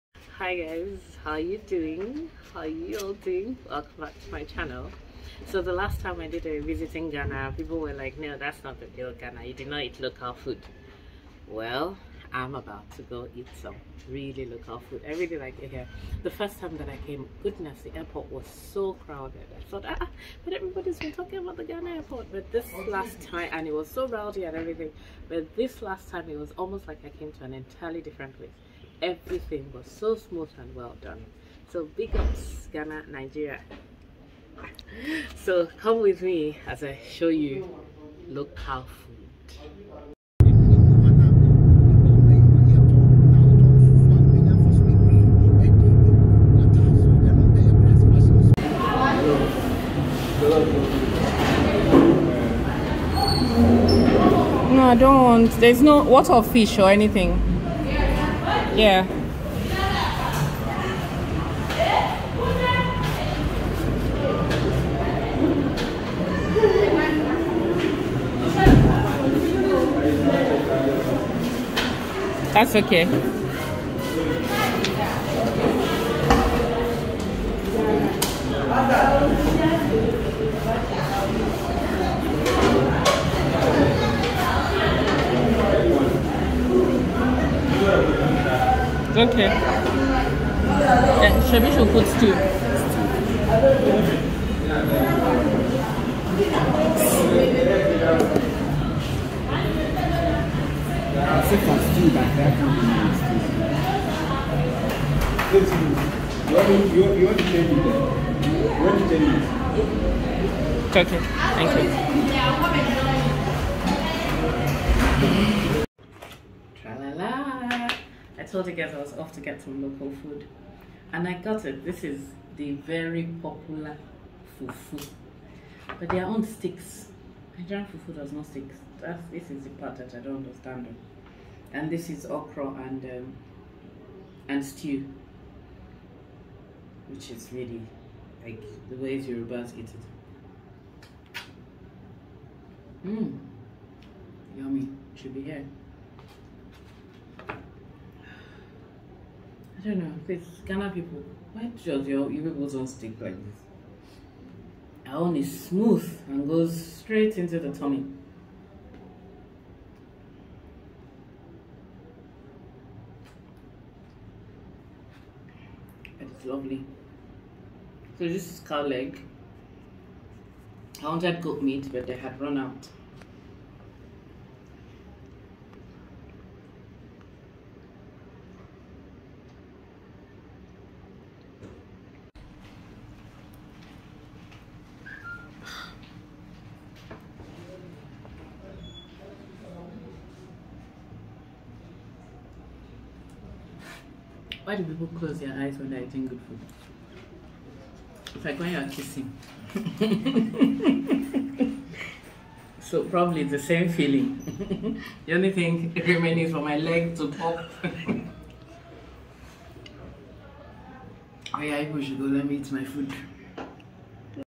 Travel Vlog : Visiting Ghana Pt. 2 - Eating Ghana food
Apparently I should have eating the fufu with palm nut soup or light soup.